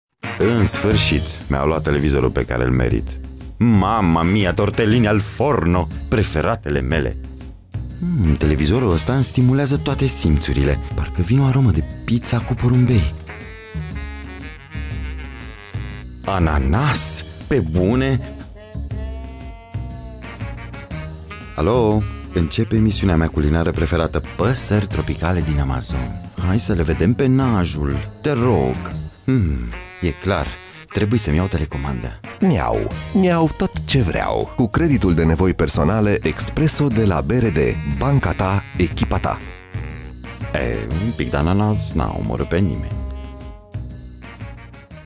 Televizorul cu Creditul Expresso de la BRD
Miau. Miau tot ce vreau! Cu Creditul de nevoi personale Expresso de la BRD. Afla toate detaliile pe